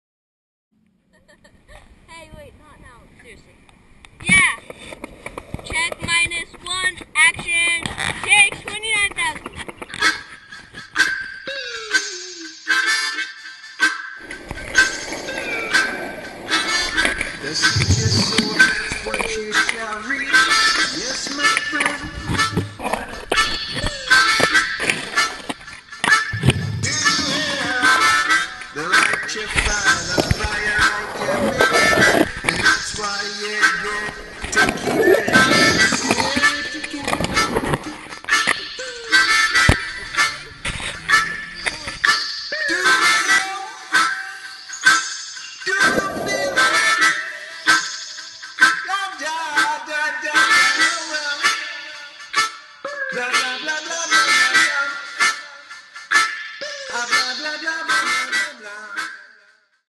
"SLAPPY SUNDAYZ" trailer - "Skater Made" Skateboards.
"Skater Made" skateboards promotes good times in skateboarding, no matter where you
are. We like the natural PMA that comes from grabbing your board and riding out
the door to get your shred on. All major competitions aside, we reserved our Sunday
for some slappies.
This is a teaser for our feature short "SLAPPY SUNDAYZ".